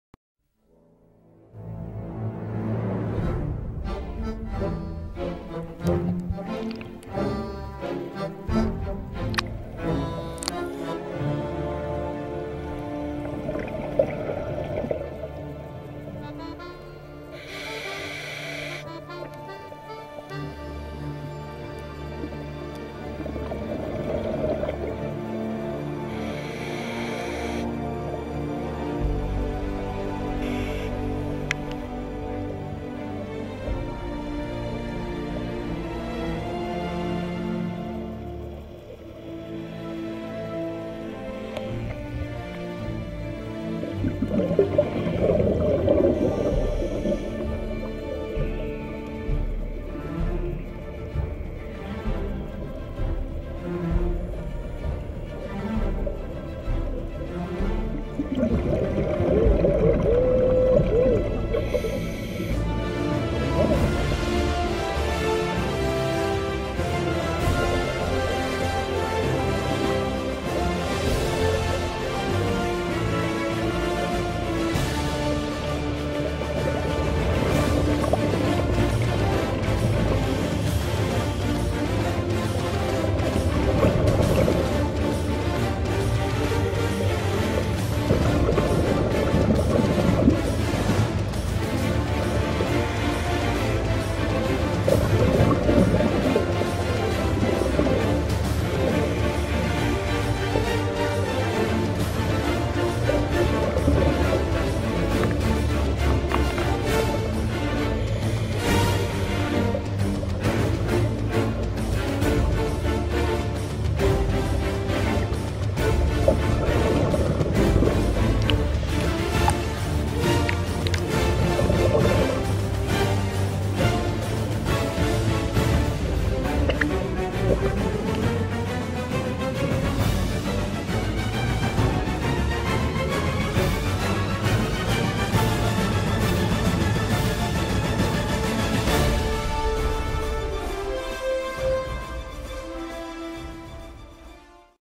OTS Full Face Course Eilat
A OTS Full face course with Eyal Sayag in Eilat Israel